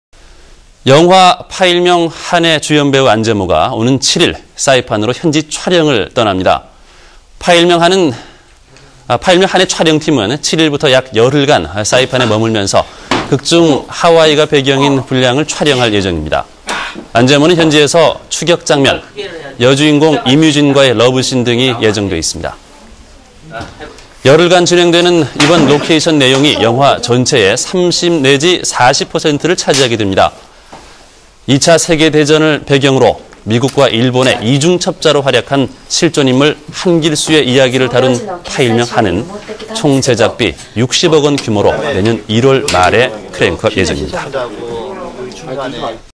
[movie] Ahn Jae-Mo 'HANN' sipan shooting (HAAN 한길수 안재모 사이판 촬영)
영화 '파일명 HANN(한)'의 주연배우 안재모가 오는 7일 사이판으로 현지 촬영을 떠납니다.

'파일명 HANN' 촬영팀은 7일부터 약 열흘 간 사이판에 머물면서 극중 하와이가 배경인 분량을 촬영할 예정입니다.

안재모는 현지에서 추격 장면, 여주인공 임유진과의 러브신 등이 예정돼있습니다. 열흘간 진행되는 이번 로케이션 내용이 영화 전체의 30-40%를 차지하게 됩니다.

2차 세계대전을 배경으로 미국과 일본의 이중첩자로 활약한 실존 인물 한길수의 이야기를 다룬 '파일명 HANN'은 총제작비 60억원 규모로 내년 1월말 크랭크 업 예정입니다.